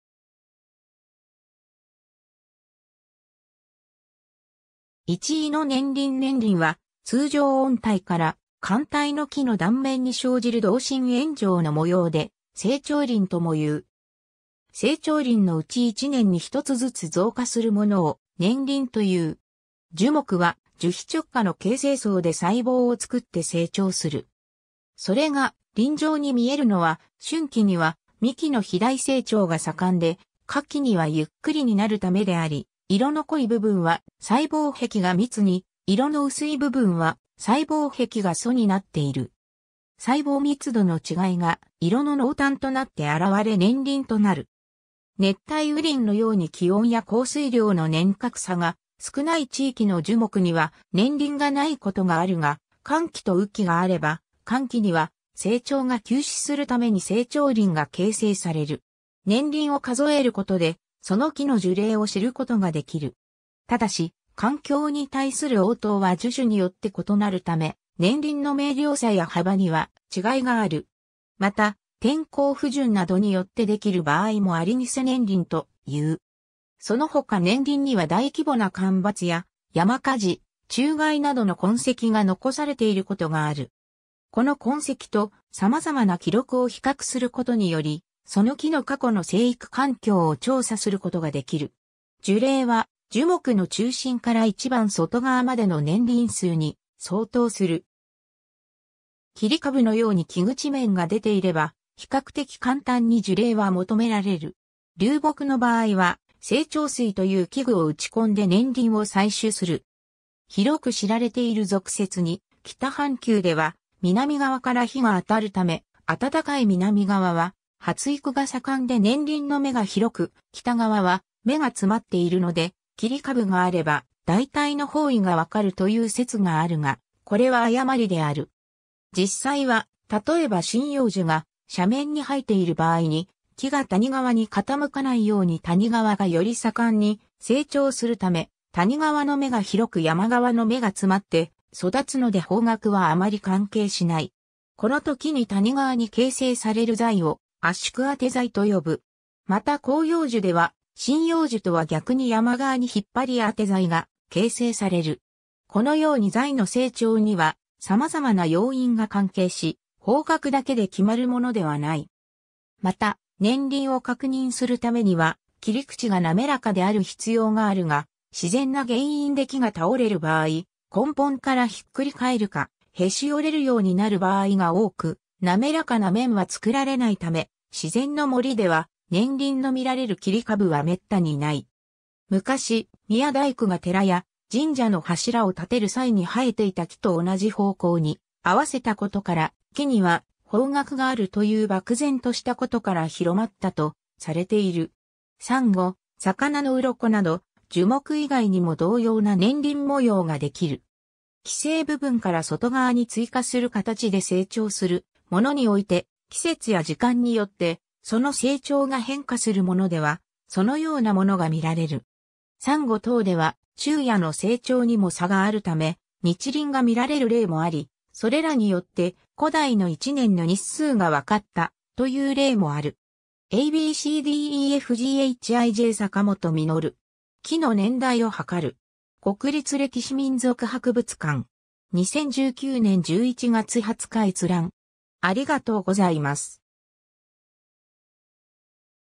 年輪
イチイの年輪 年輪（ねんりん、growth ring）は、通常温帯から寒帯の木の断面に生じる同心円状の模様で、成長輪ともいう。
成長輪のうち1年に一つずつ増加するものを年輪という。
 樹木は樹皮直下の形成層で細胞を作って成長する。
それが輪状に見えるのは、春期には幹の肥大成長が盛んで、夏期にはゆっくりになるためであり、色の濃い部分は細胞壁が密に、色の薄い部分は細胞壁が疎になっている。
細胞密度の違いが色の濃淡となって現れ年輪となる。
熱帯雨林のように気温や降水量の年較差が少ない地域の樹木には年輪がないことがあるが、乾季と雨季があれば乾季には成長が休止するために成長輪が形成される。
 年輪を数えることで、その木の樹齢を知ることができる。
ただし、環境に対する応答は樹種によって異なるため年輪の明瞭さや幅には違いがある。
また、天候不順などによってできる場合もあり偽年輪という。
そのほか年輪には大規模な旱魃や山火事、虫害などの痕跡が残されていることがある。
この痕跡と様々な記録を比較することにより、その木の過去の生育環境を調査することができる。
 樹齢は樹木の中心から一番外側までの年輪数に相当する。
切り株のように木口面（年輪面）が出ていれば比較的簡単に樹齢は求められる。
立木の場合は生長錐という器具を打ち込んで年輪を採取する。
 広く知られている俗説に、「北半球では南側から日が当たる為、暖かい南側は発育が盛んで年輪の目が広く、北側は目が詰まっているので、切り株があれば大体の方位がわかる」という説があるが、これは誤りである。
実際は、たとえば針葉樹が斜面に生えている場合に、木が谷側に傾かないように谷側がより盛んに成長する為、谷側の目が広く山側の目が詰まって育つので方角はあまり関係しない。
この時に谷側に形成される材を圧縮あて材と呼ぶ。
また広葉樹では針葉樹とは逆に山側に引っ張りあて材が形成される。
このように材の成長には様々な要因が関係し、方角だけで決まるものではない。
また、年輪を確認するためには切り口が滑らかである必要があるが、自然な原因で木が倒れる場合、根本からひっくり返るか、へし折れるようになる場合が多く、滑らかな面は作られないため、自然の森では年輪の見られる切り株は滅多にない。
昔、宮大工が寺や神社の柱を建てる際に生えていた木と同じ方向に合わせたことから「木には方角がある」という漠然としたことから広まったとされている。
 サンゴ、魚の鱗など樹木以外にも同様な年輪模様ができる。
既製部分から外側に追加する形で成長するものにおいて、季節や時間によってその成長が変化するものではそのようなものが見られる。
サンゴ等では昼夜の成長にも差があるため、日輪が見られる例もあり、それらによって、古代の一年の日数が分かった、という例もある。
 ^ a b c d e f g h i j 坂本 稔. “木の年代をはかる”. 国立歴史民俗博物館. 2019年11月20日閲覧。